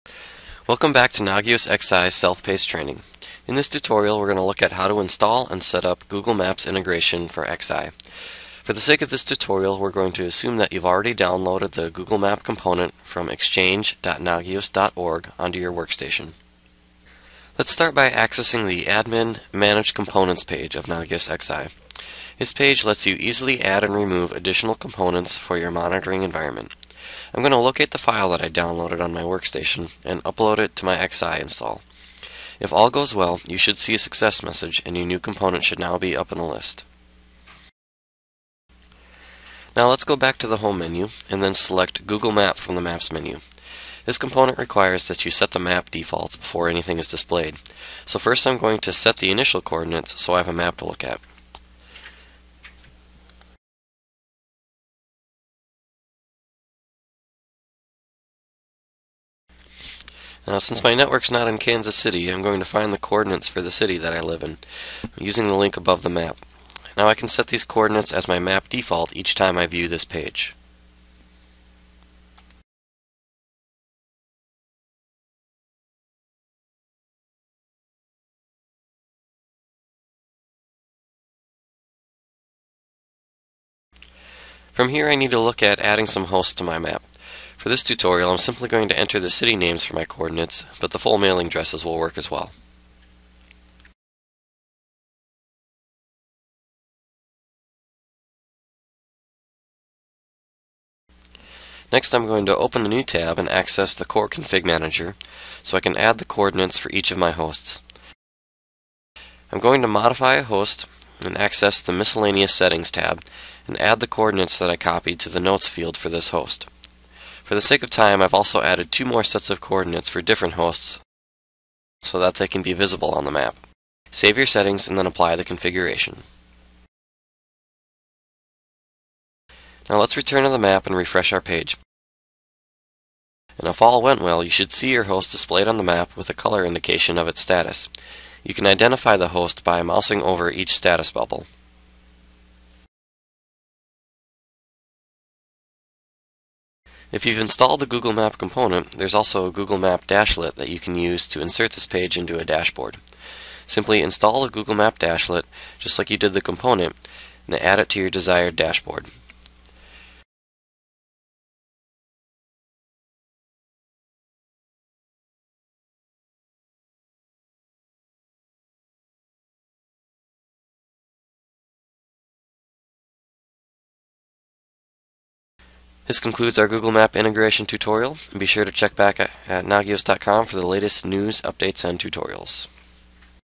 Icinga Google Maps Integration
This video shows you how to integrate Google Maps with Icinga.